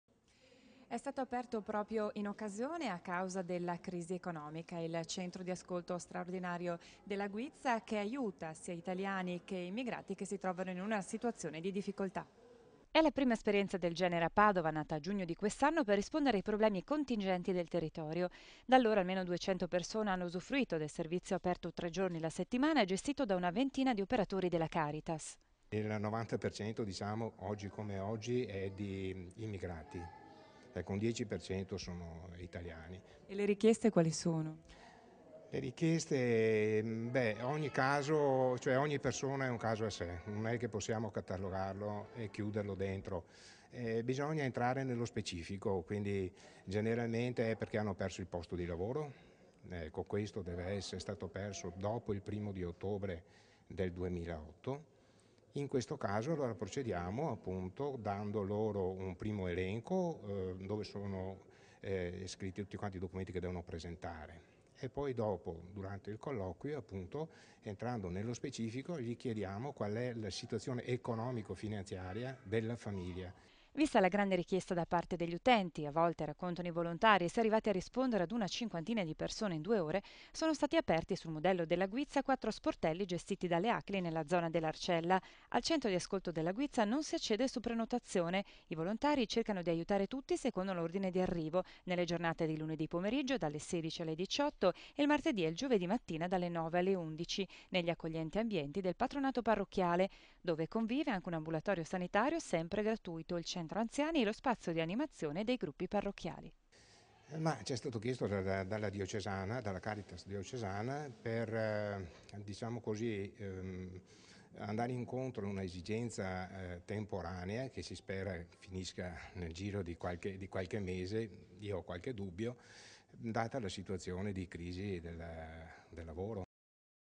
091013 Un centro straordinario di ascolto dopo la crisi
Padova - Da giugno � attivo un centro di ascolto straordinario alla Guizza nnato in seguito alla crisi gestito dalla Caritas (Micaela Faggiani)